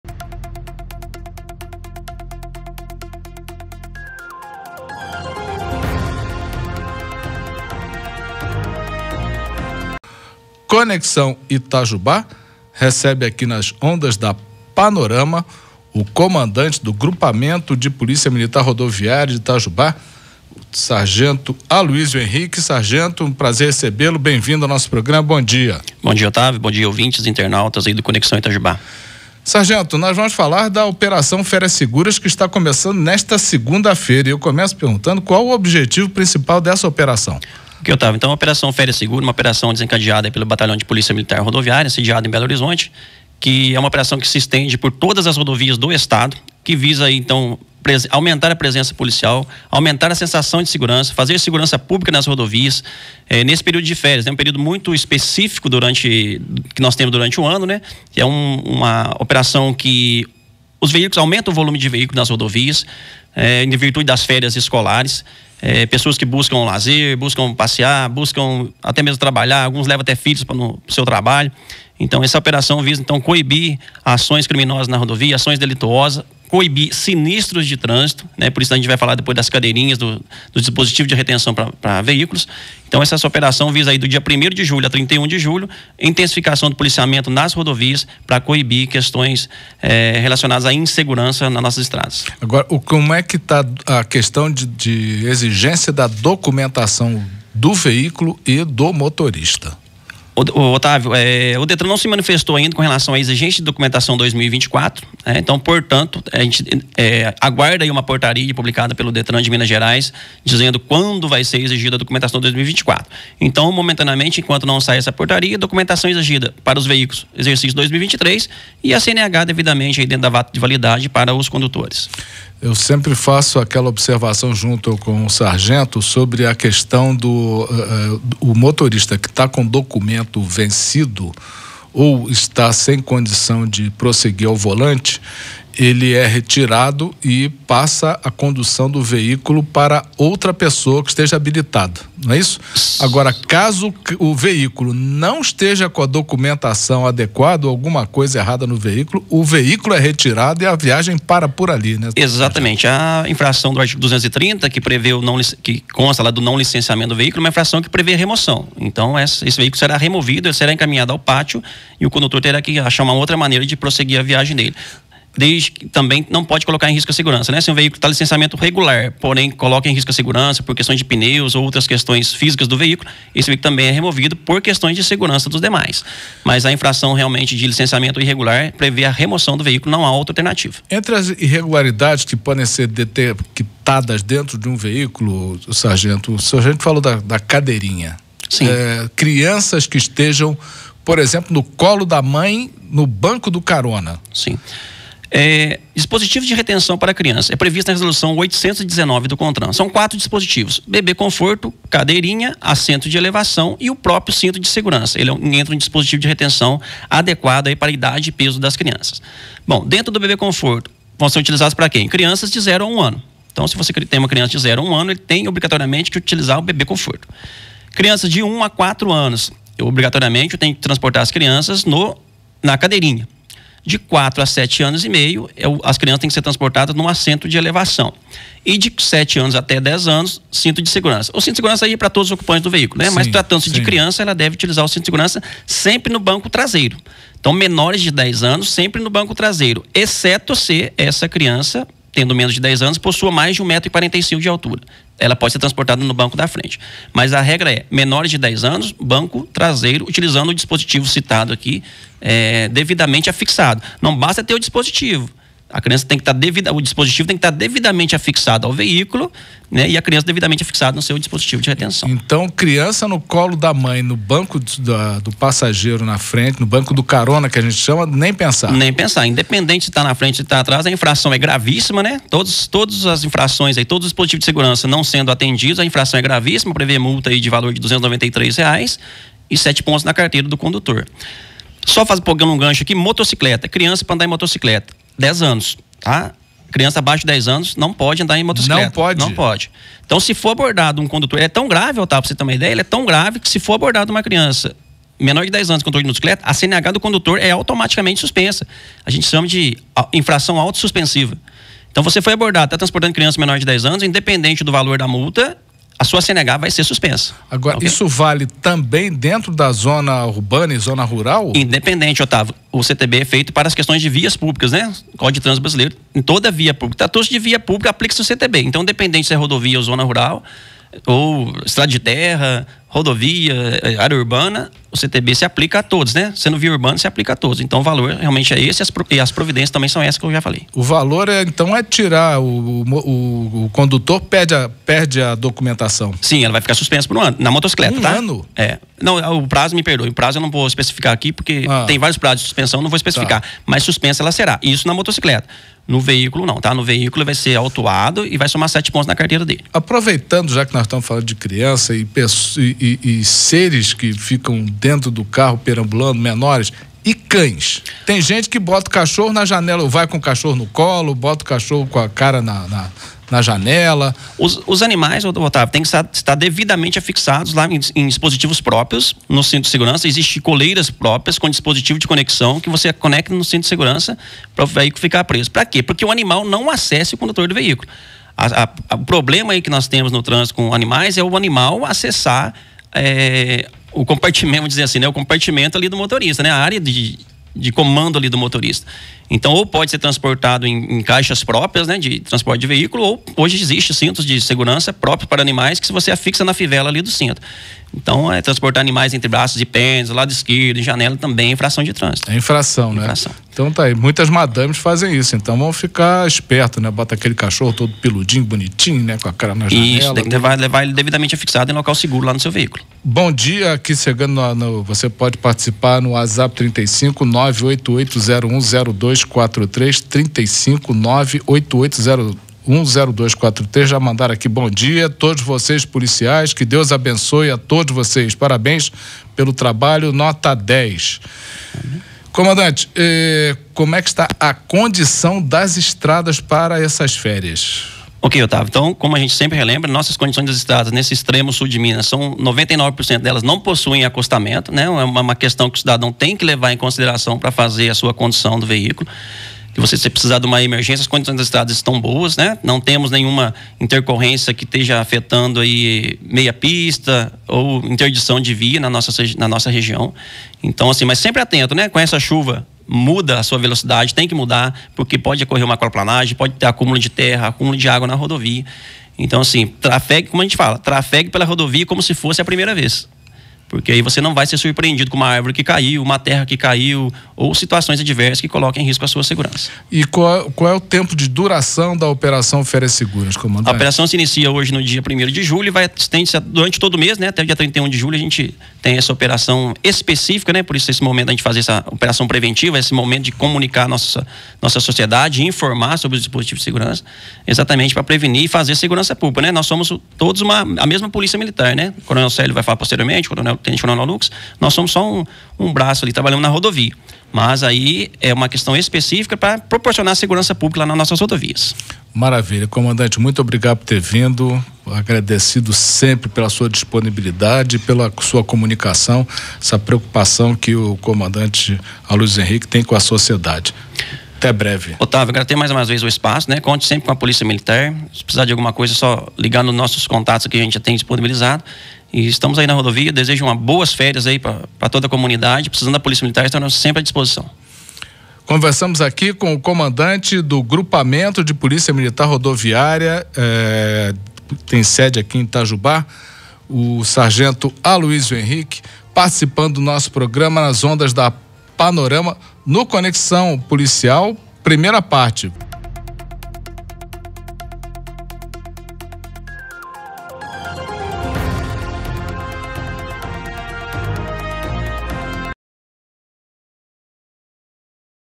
PM Rodoviária anuncia início da Operação Férias Seguras
O comandante do 1º Grupamento da Polícia Militar Rodoviária, Sargento Aloísio Henrique, detalhou os objetivos da operação e forneceu orientações sobre a documentação necessária, equipamentos de segurança para crianças e as condições das estradas.